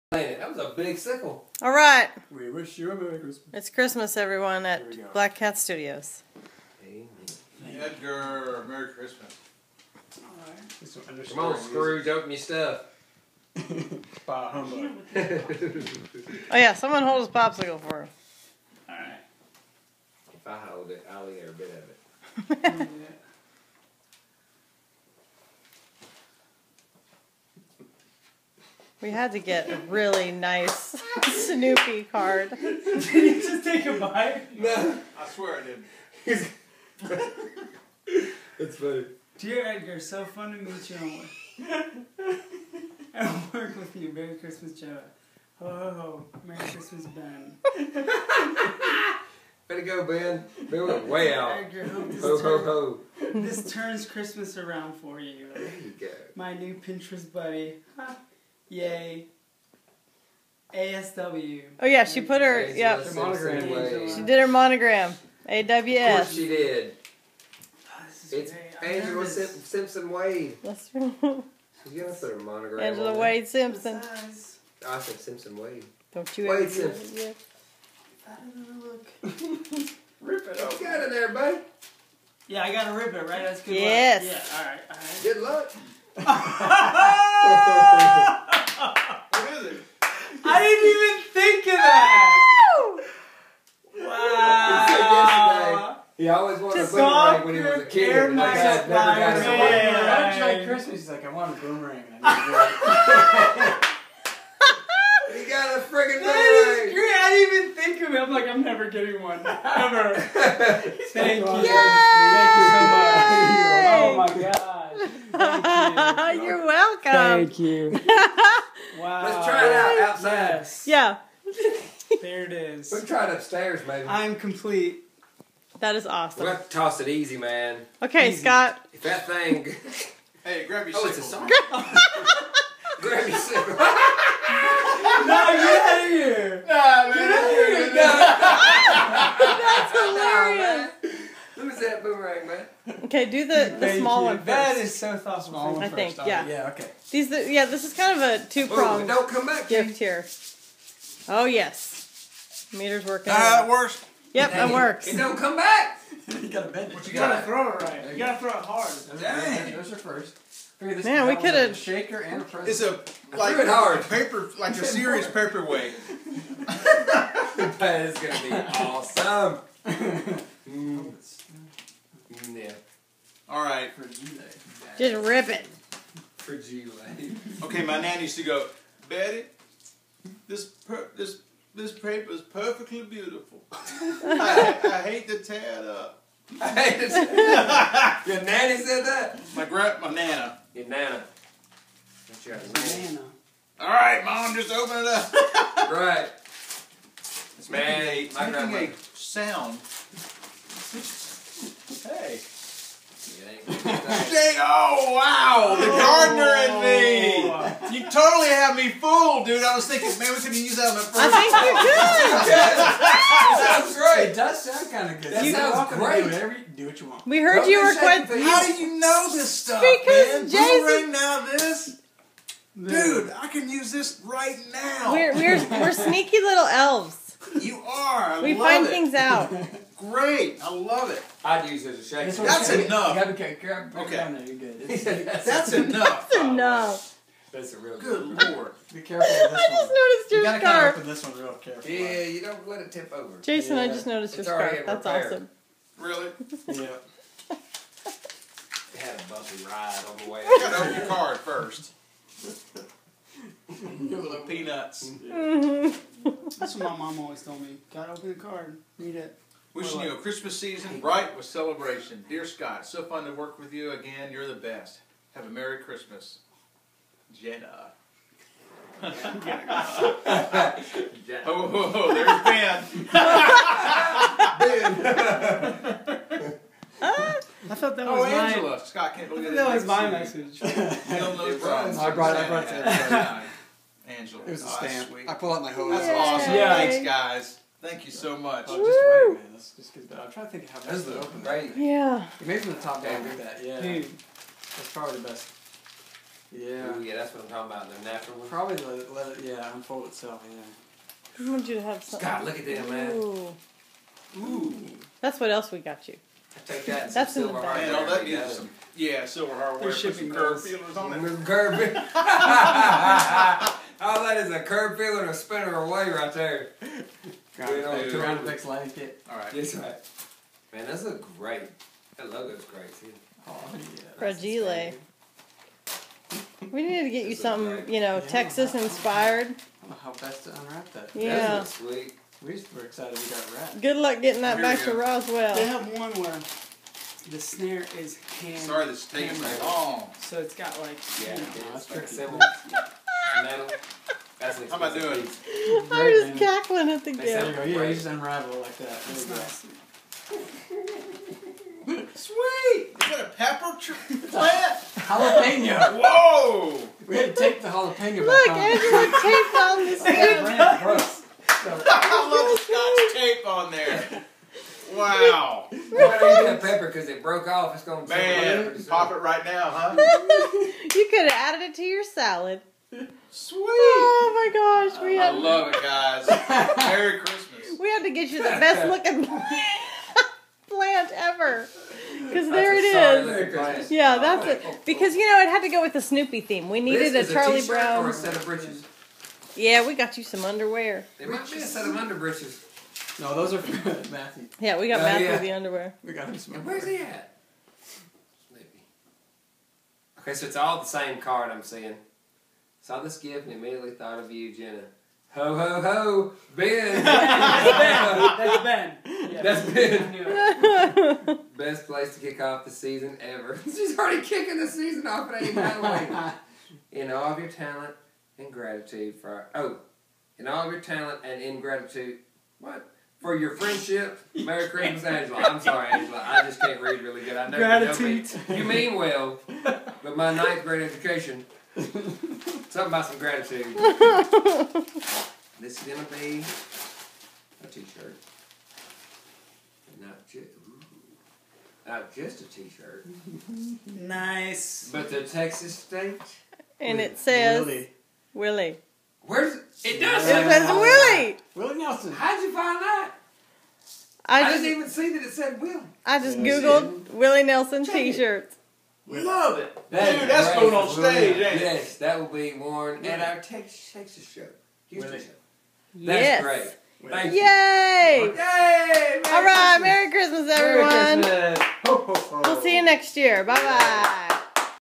Christmas in September at Black Cat Studios
Edgar finally gets his boomerang, thereby redeeming Christmas for him forever. Scott the green thumb and Dang Dean of Drums gets a silly Texas jalapeño plant, and Hoard gets a Willie shirt. Yellow Fence music loves some BCS!